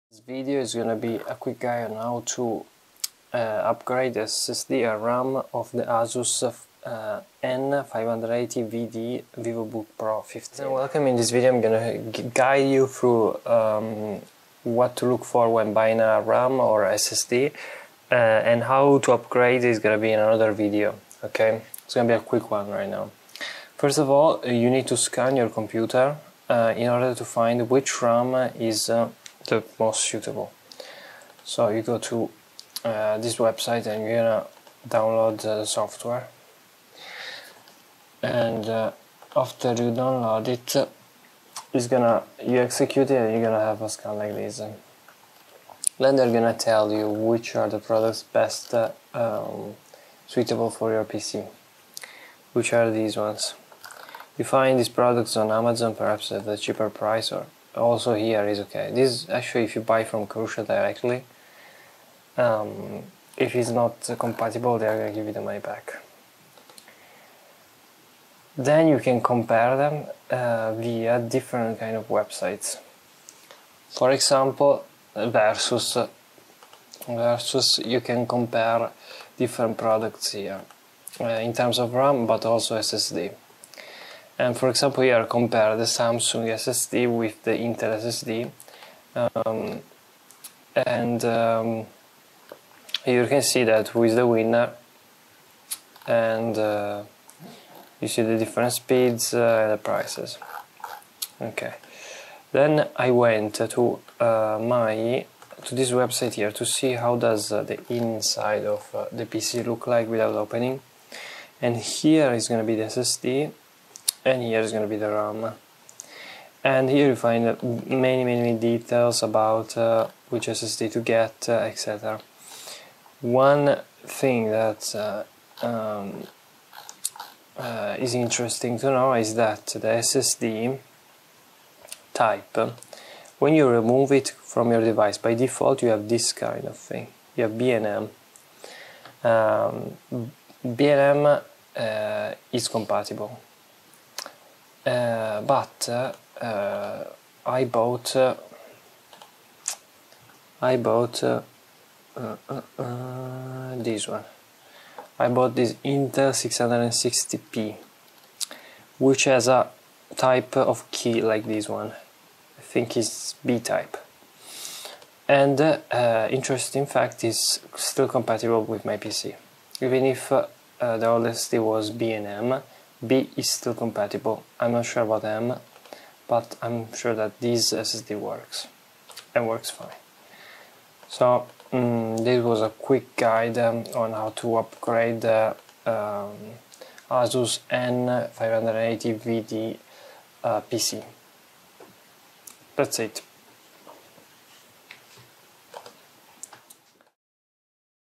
Asus n580vd Vivobook Pro 15: How to Chose a RAM and SSD for UPGRADE
Great article(with pictures) on the hardware of Asus n580vd vivobook pro 15 hardware